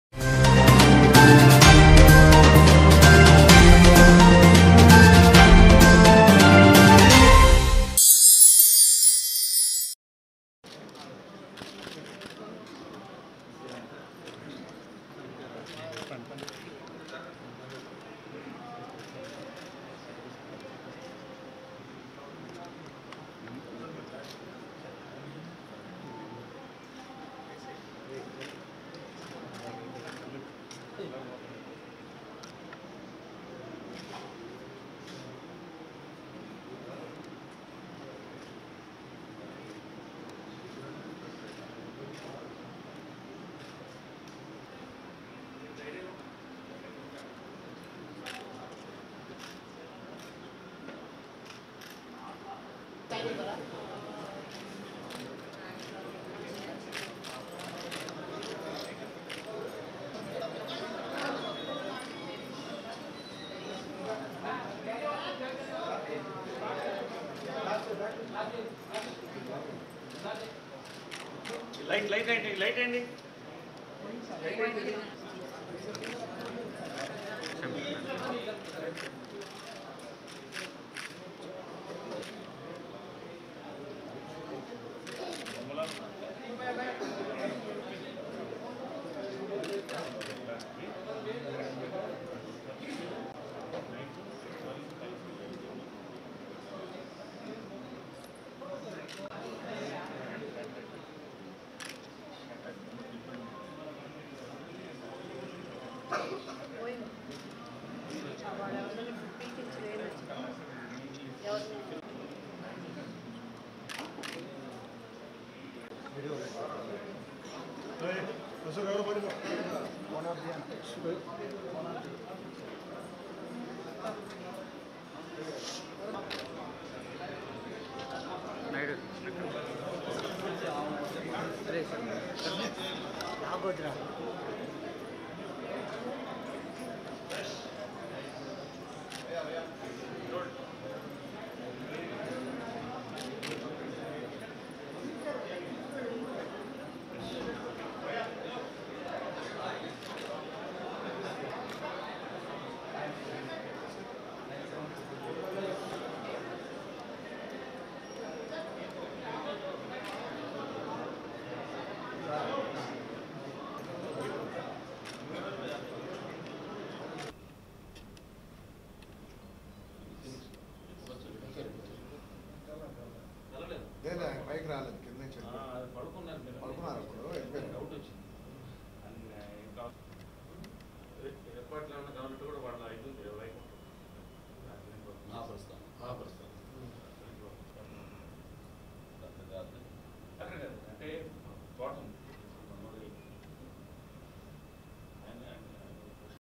Vijaya Nirmala Passes Away || విజయనిర్మల మృతితో కన్నీటి పర్యంతమైన కృష్ణ, మహేష్ బాబు || telugunewstv
Like and follow @TeluguNewsTV, The News channel with exclusive Breaking news, Special Interviews, latest updates on Politics, Movies, Sports and Current Affair